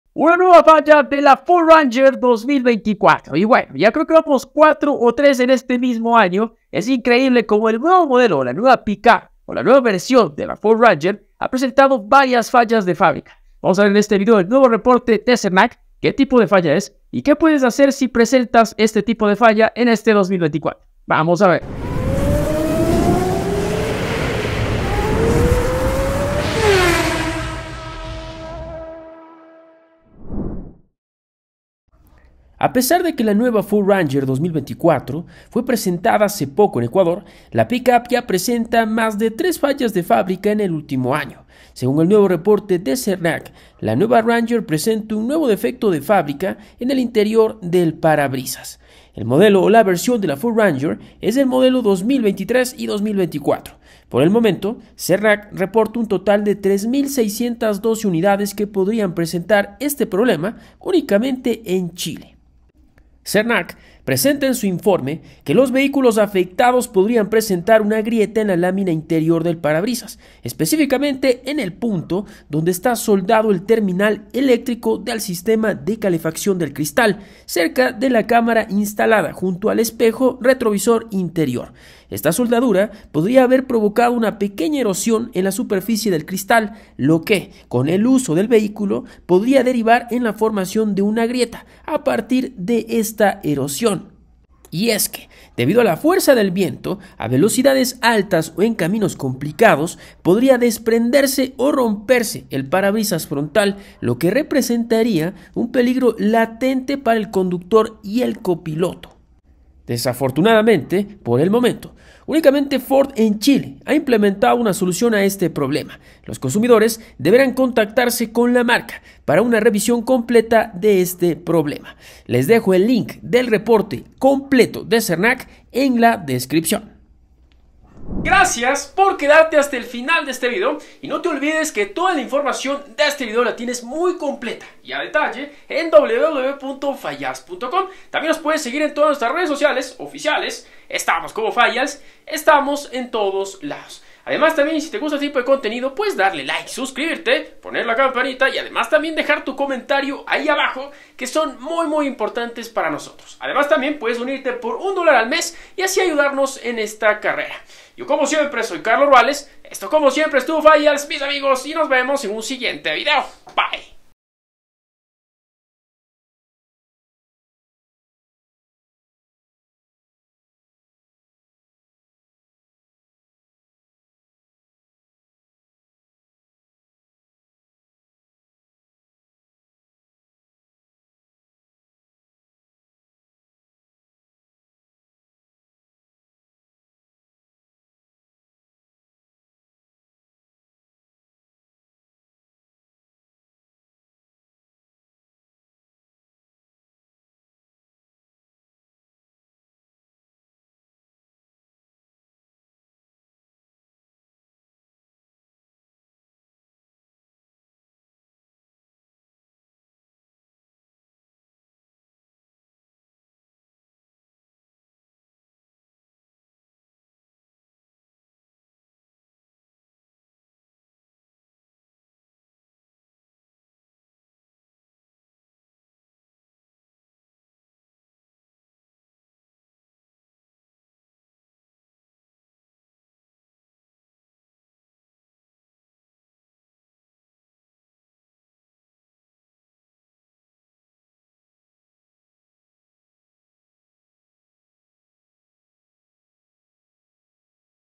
¿Tu Ford Ranger 2024 tiene este problema? 🤔 ¡Descúbrelo ahora!
¿Tienes una Ford Ranger 2024?  ¡Atención! En este video te revelamos una falla que podría estar afectando tu camioneta. ⚠️ Recientemente se ha descubierto un problema de fabricación que podría causar [describir brevemente la falla, por ejemplo: "grietas en el parabrisas", "fallos en el sistema de frenos", etc.].

Te explicaremos en detalle qué es esta falla, cuáles son sus consecuencias y cómo puedes identificar si tu Ranger está afectada. Además, te daremos consejos sobre qué hacer si descubres que tienes este problema. ¡No te quedes sin saber!

La portada o miniatura del video es una gráfica representativa con humor, no se asemeja a la realidad.